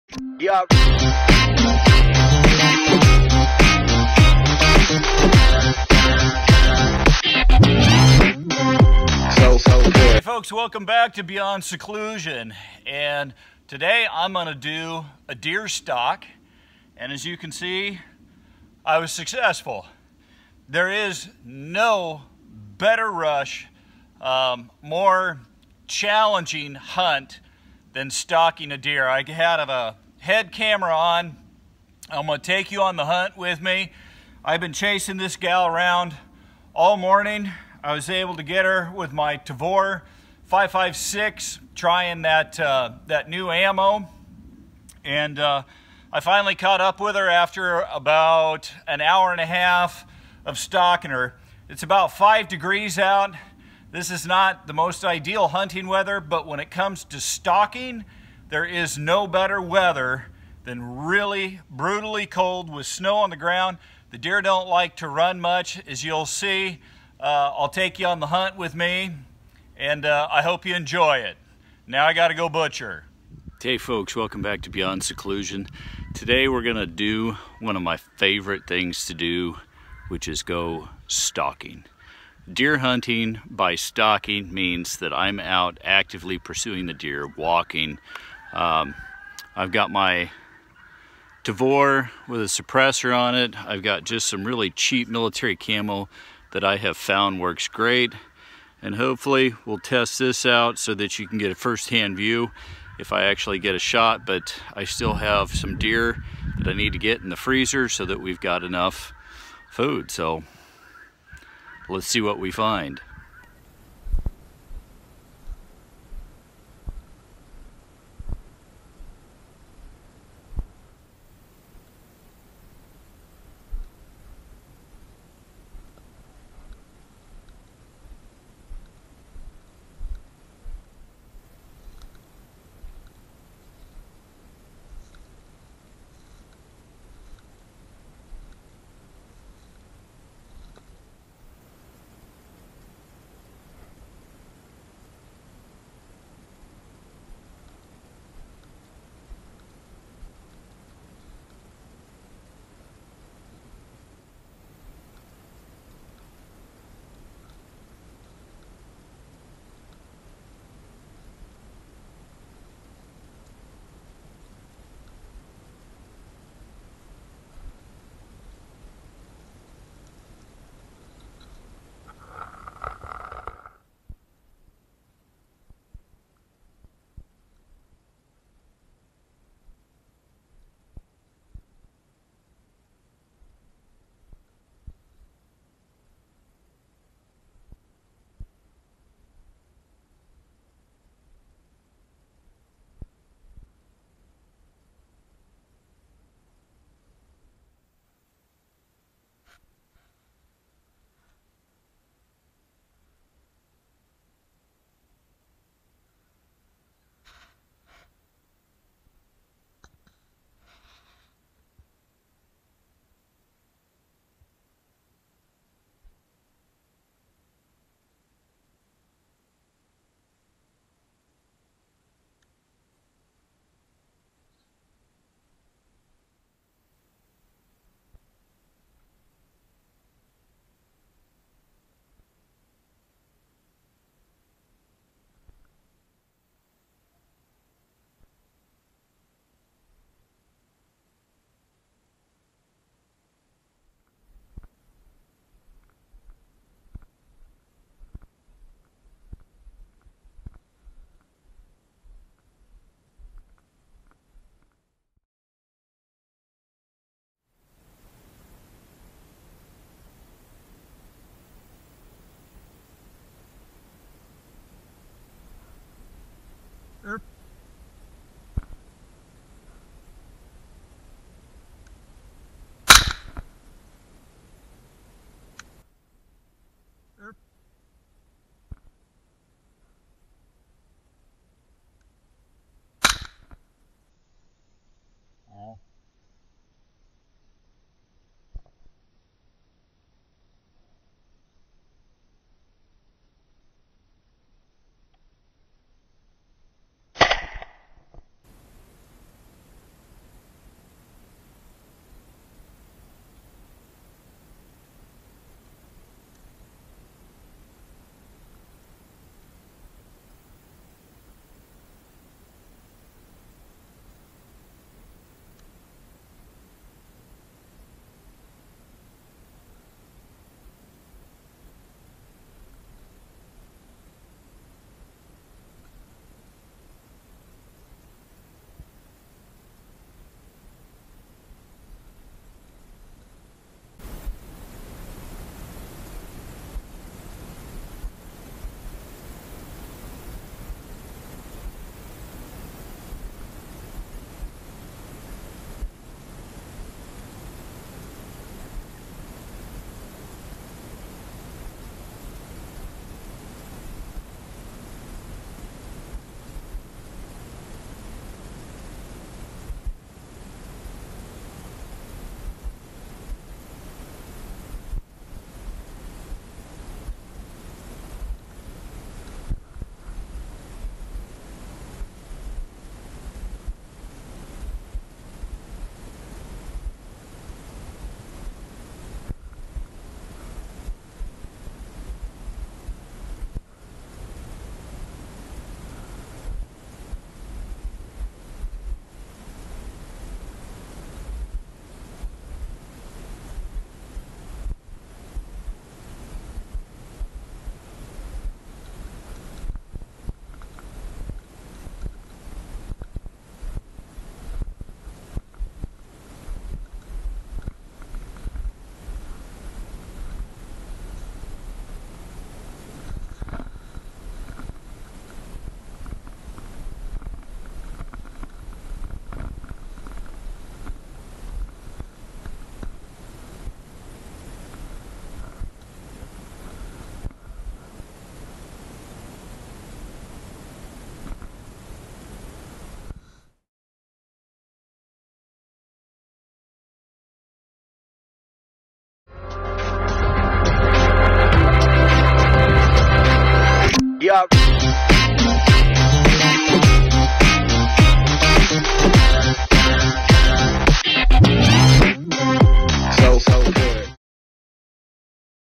Deer Stalk - Hunting at Beyond Seclusion
It took really cold weather to catch up and sneak up on these deer.  I was trying a new .223 round from my IWI Tavor with Meprolite optic.  This is the BIGGEST doe I have ever shot!
If you enjoyed this, please share with friends.

Drew